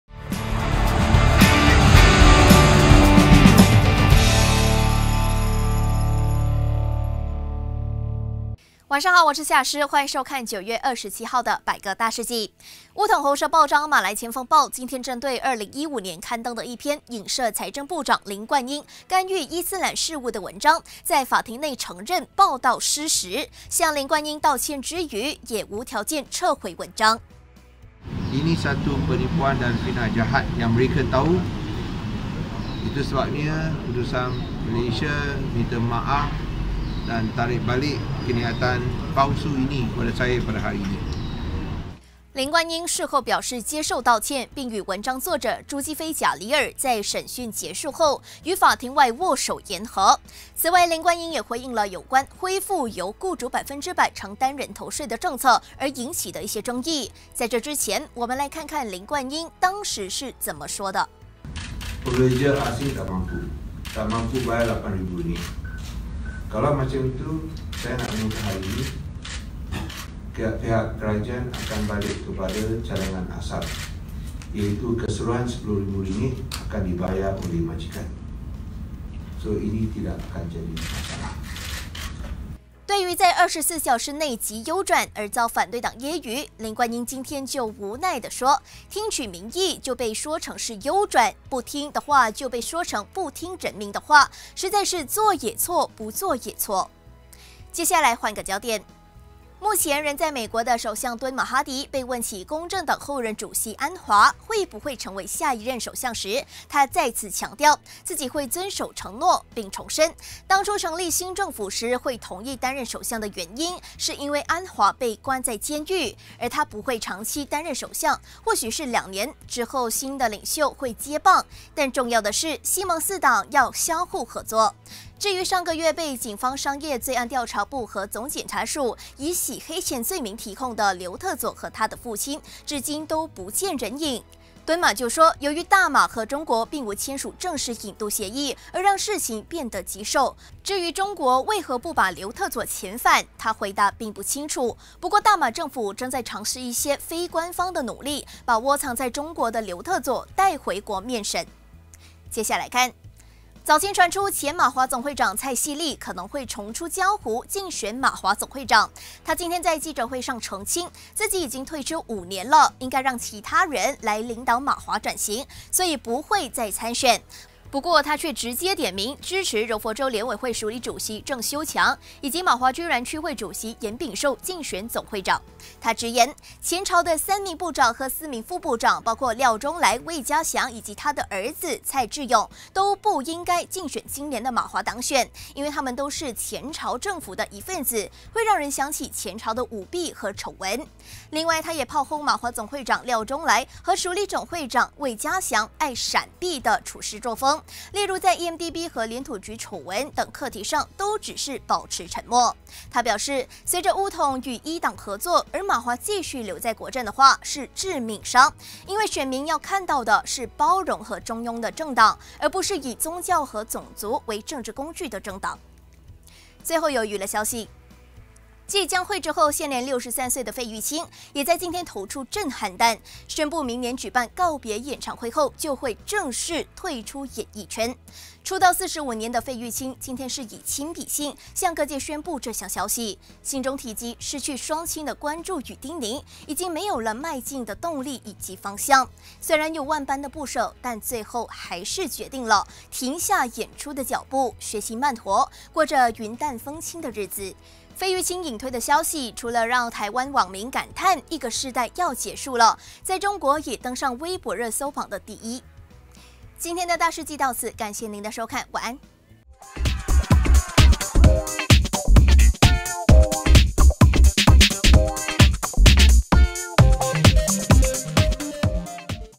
将刘特佐带回国面审 敦马：尝试“非官方行动” |大事纪
刘特佐确定匿藏中国，大马政府真的拿他没办法？敦马受访时透露，目前正尝试“非官方”行动将刘特佐带回国面审.